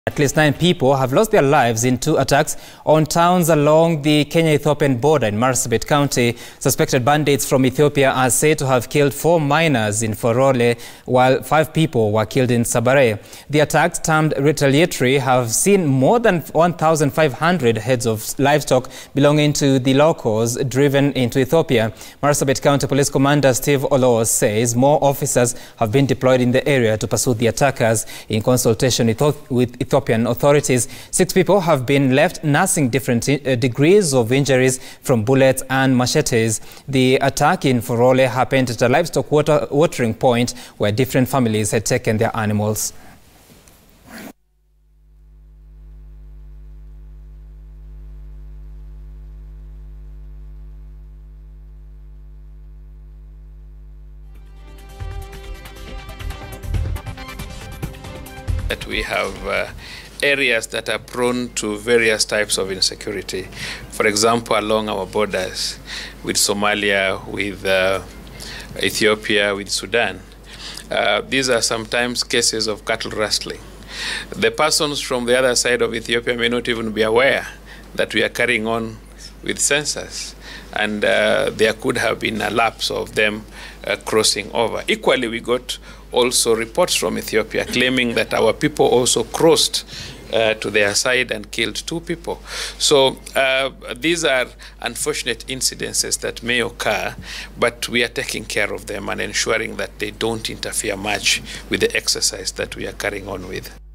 9 killed in two separate attacks in Marsabit
At least nine people lost their lives in two attacks on towns along the Kenya Ethiopian border in Marsabit County. Suspected bandits from Ethiopia are said to have killed four minors in Forole while five people were killed in Sabarreh.